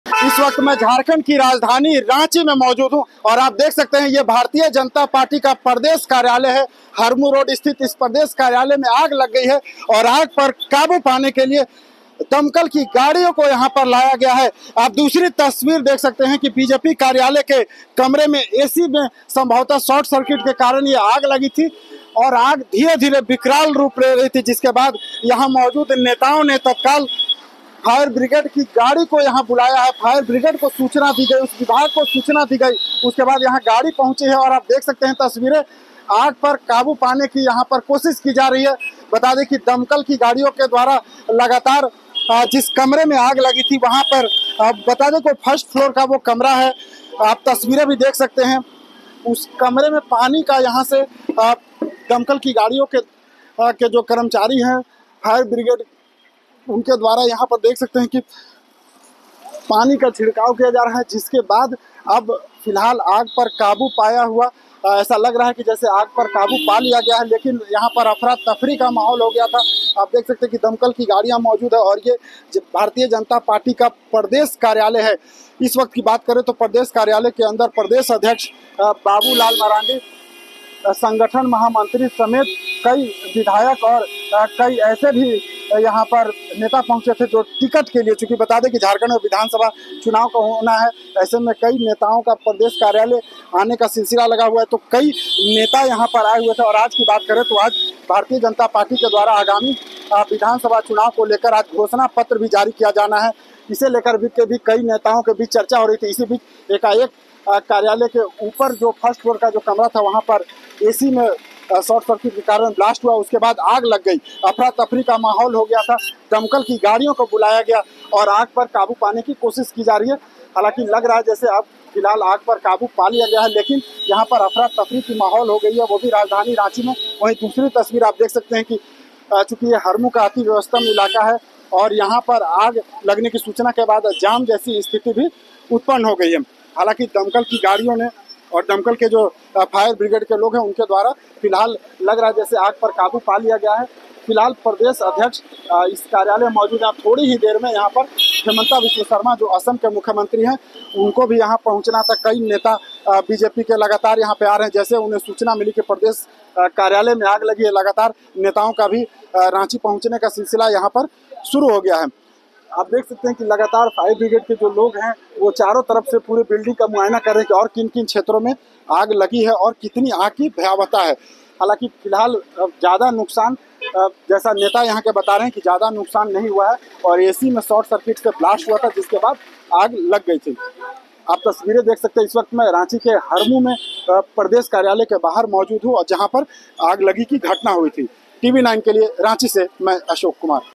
Jharkhand BJP प्रदेश मुख्यालय में शॉर्ट सर्किट और एसी में ब्लास्ट से लगी आग.. हालात काबू में।
झारखंड बीजेपी प्रदेश मुख्यालय के फर्स्ट फ्लोर के कमरे में शॉर्ट सर्किट और एसी में ब्लास्ट के कारण लगी आग.. दमकल विभाग ने आग पर काबू पाया.. झारखंड बीजेपी के प्रदेश अध्यक्ष बाबूलाल मरांडी समेत कई नेता और विधायक प्रदेश कार्यालय में घटना के वक्त मौजूद थे..